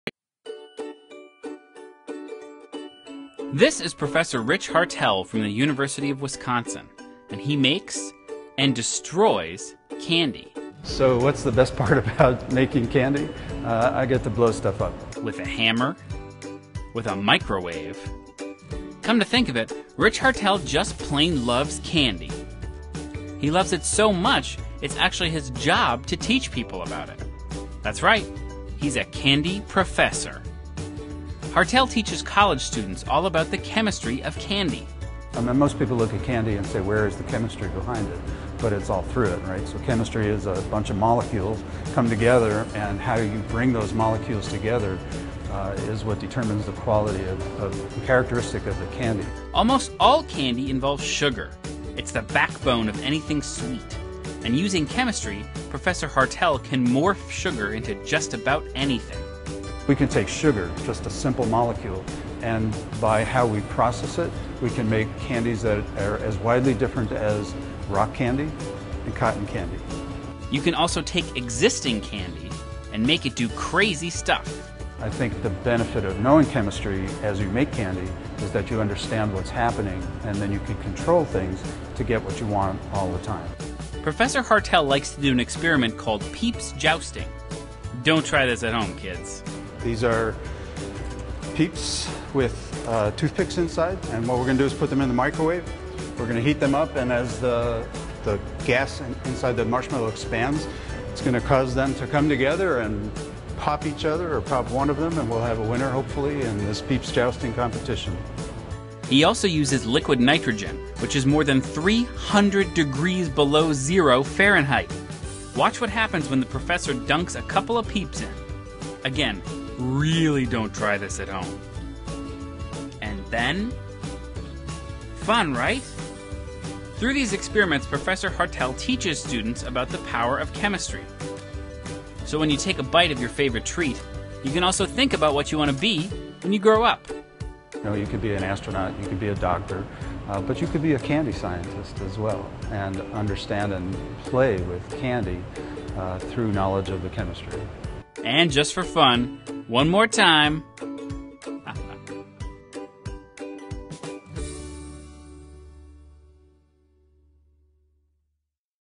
Candy Chemistry with Rich Hartel, Ph.D. - National Chemistry Week
Watch as Professor Rich Hartel, Ph.D., from the University of Wisconsin- Madison microwaves and smashes his way through the chemistry of candy!